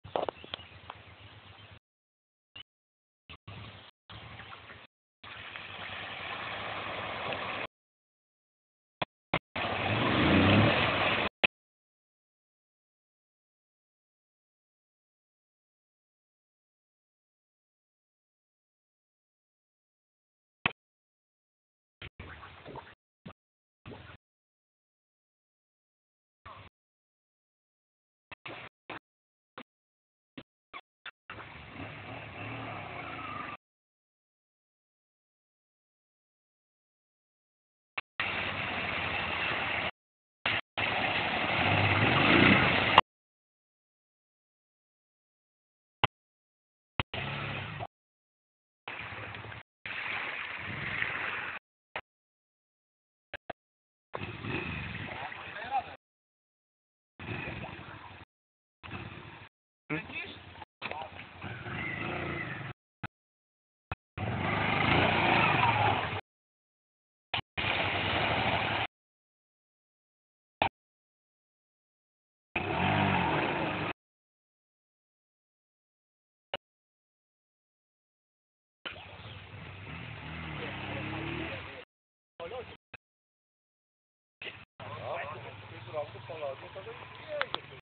transalp 650 jazda w wodzie Orawka 2010 transalp in water off road 2
dalszy ciag eksperymentow z jazda transalpem w wodzie. swietna zabawa:-)